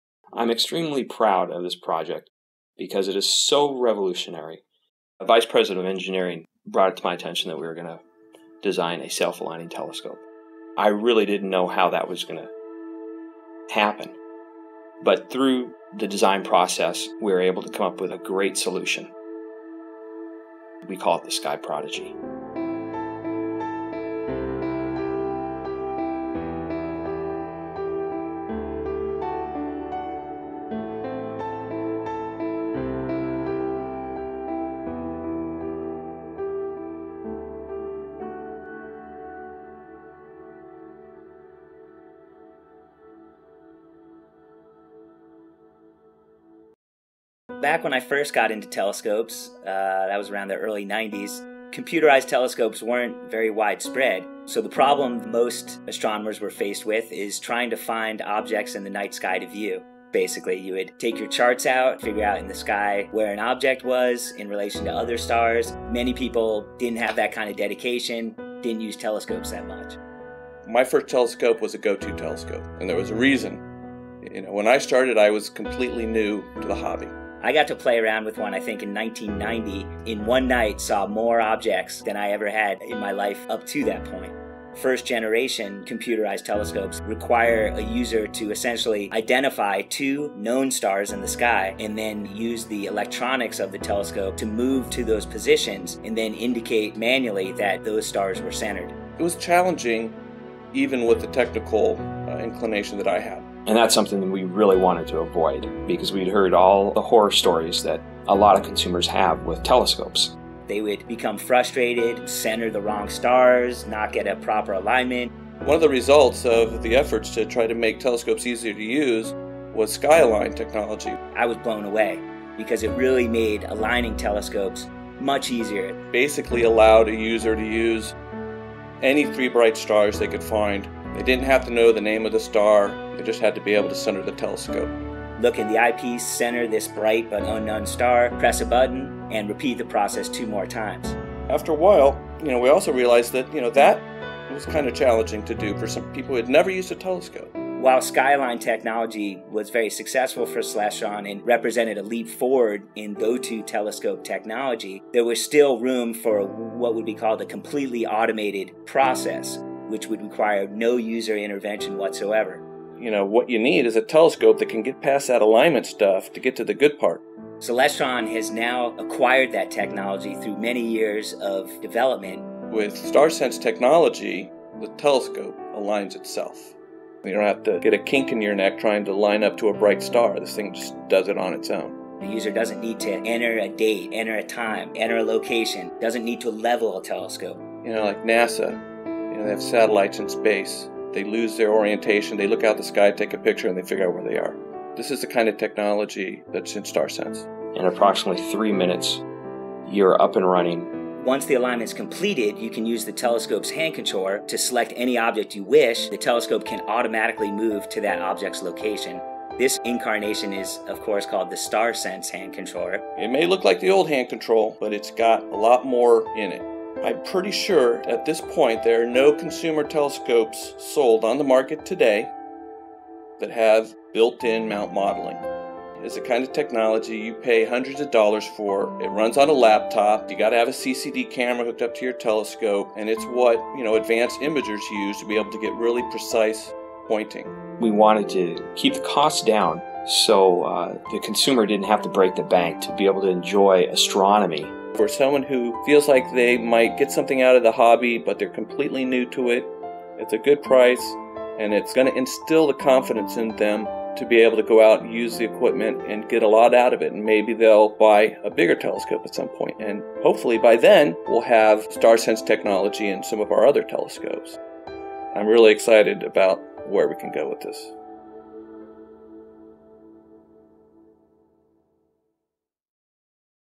SkyProdigy: The Origins
Learn about the origins of SkyProdigy directly from the engineering team responsible for designing this groundbreaking new telescope